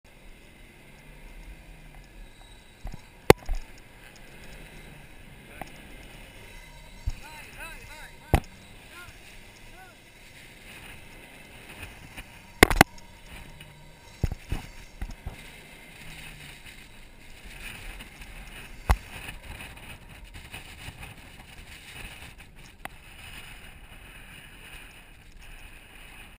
Jeep throught the desert
Climbing dunes with Jeep Grand Cherokee wk/wh 2007. One of the best holiday of my life.